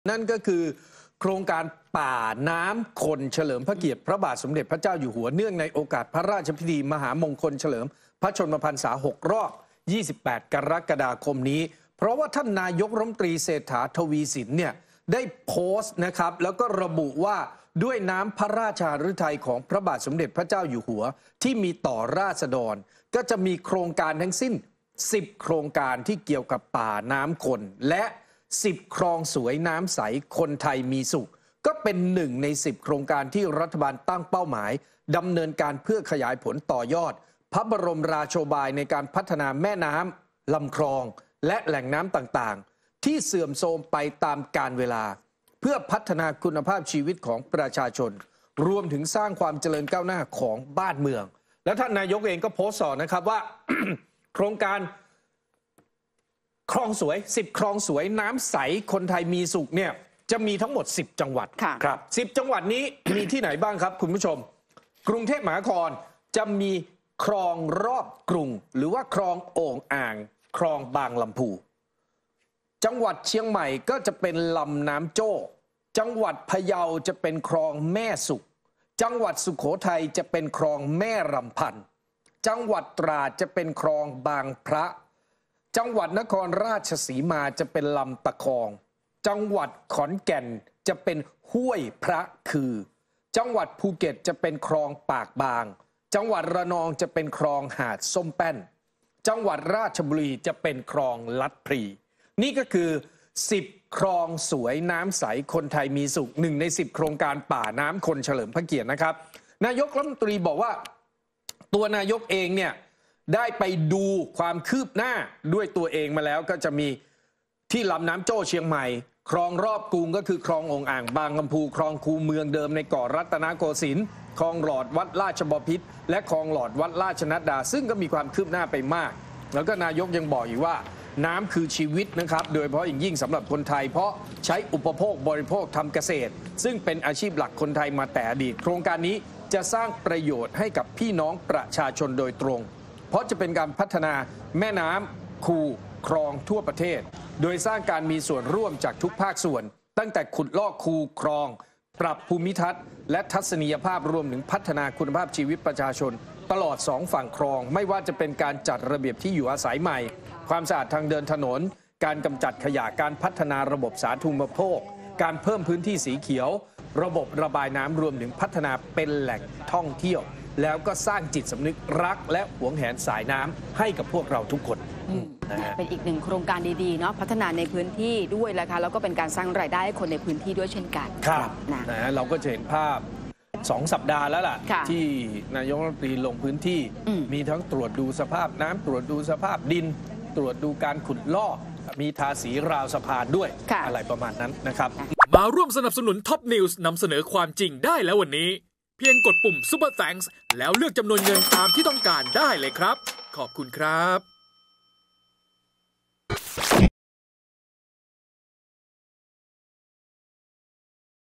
"นายกฯ" เผย รัฐบาลเดินหน้าโครงการ “10 คลองสวย น้ำใส คนไทยมีสุข”
"นายกฯ" เผย รัฐบาลเดินหน้าโครงการ “10 คลองสวย น้ำใส คนไทยมีสุข” ขยายผลต่อยอดพระบรมราโชบาย พัฒนาคุณภาพชีวิตของประชาชน